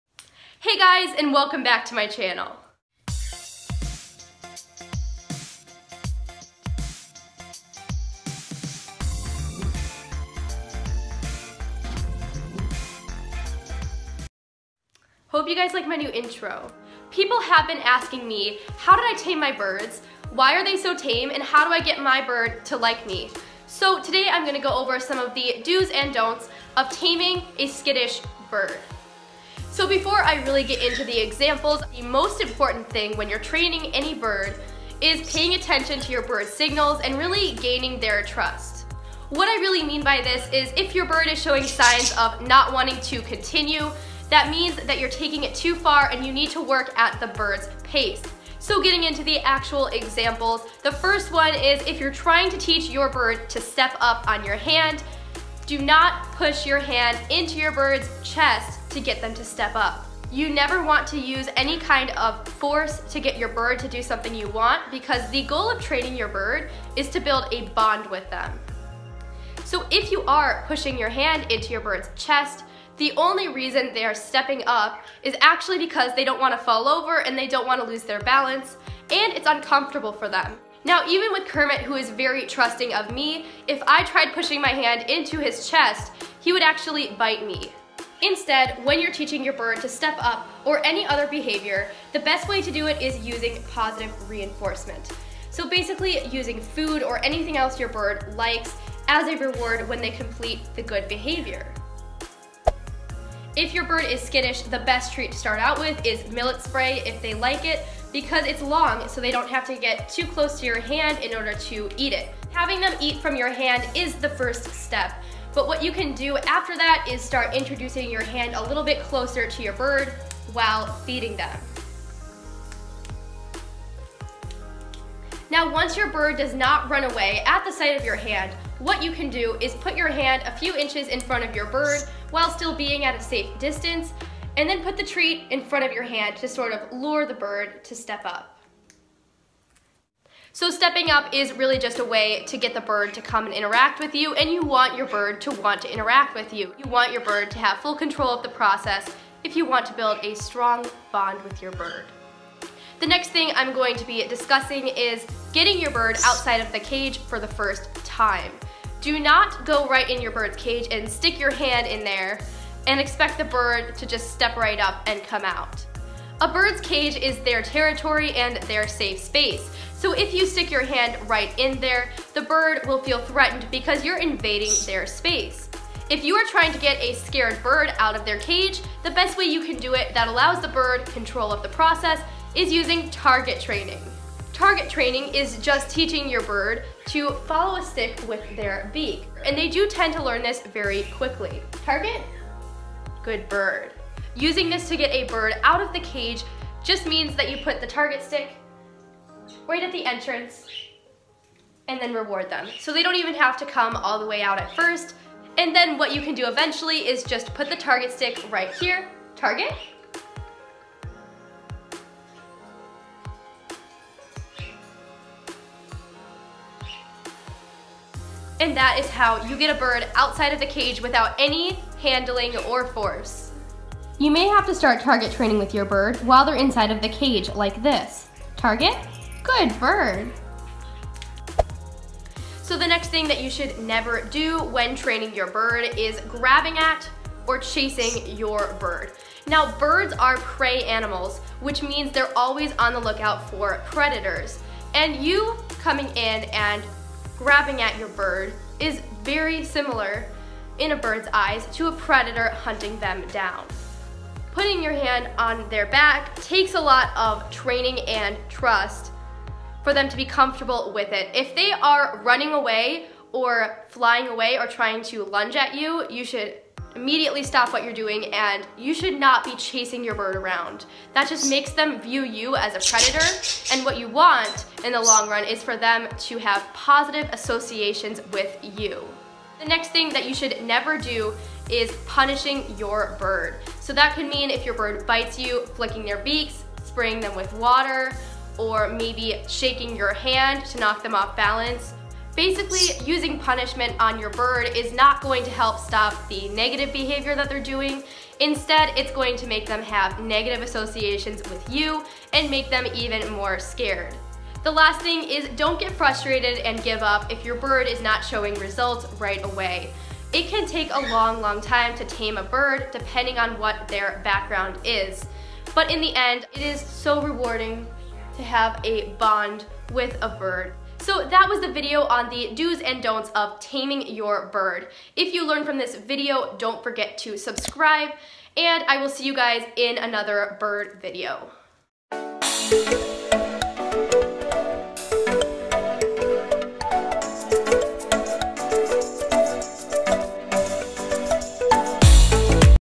What NOT to Do When Training a Scared Bird and What to Do Instead! | How to Tame a Parrot
Many of you guys have been asking me to do a video on taming birds, so here it is! Here are some of the do’s and don’ts for taming a skittish bird.

🦜MY FLOCK🦜
💙Bluebell - sky blue budgie
💛Ducky - lutino cockatiel
💚Kermit - yellow-sided green cheek conure
🤍Pearl - albino budgie